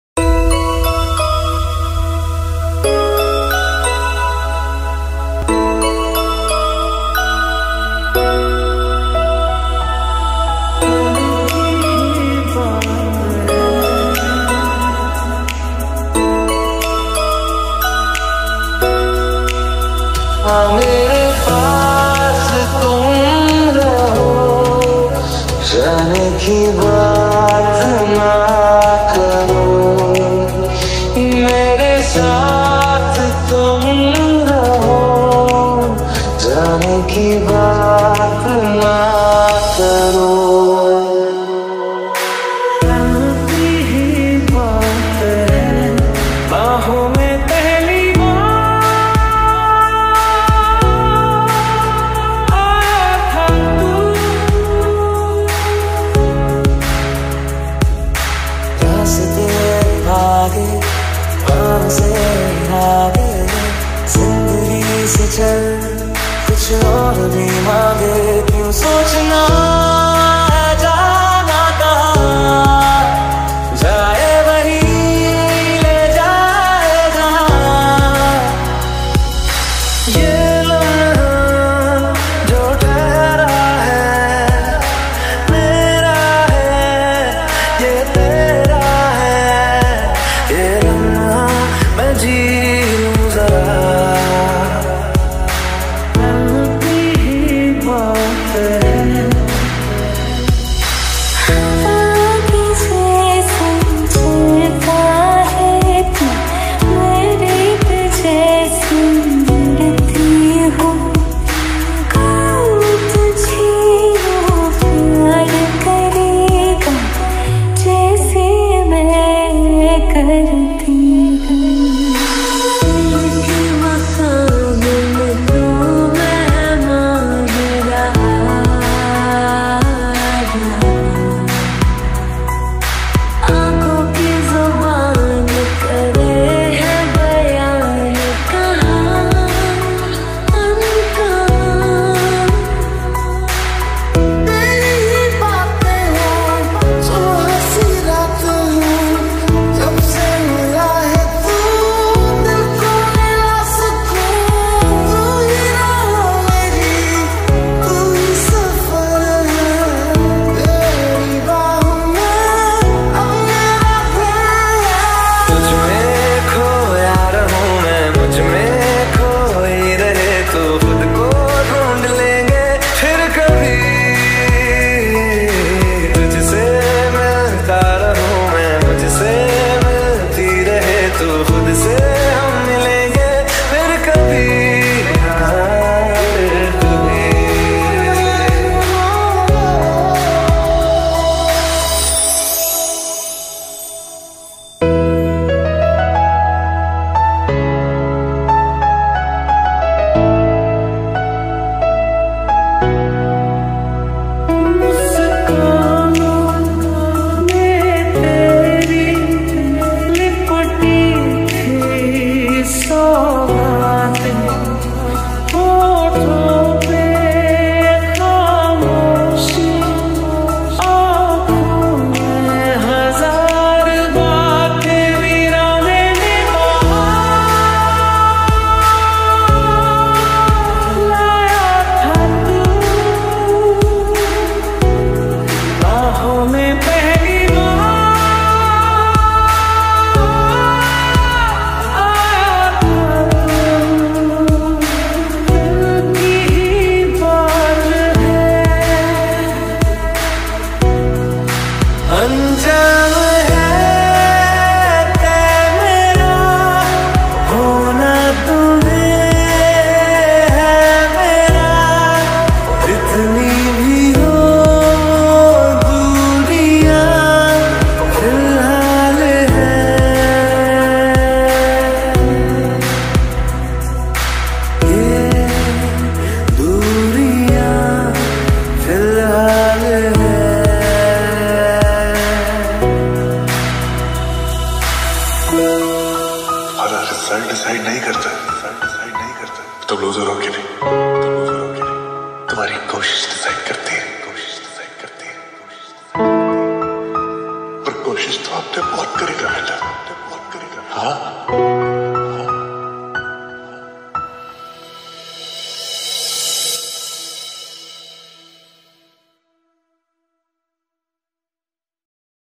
Sushant Singh Rajput Mashup - YT World | 3D Audio | Funky Nation
Tribute mashup to Sushant Singh Rajput
Hit Songs Of Sushant Singh Rajput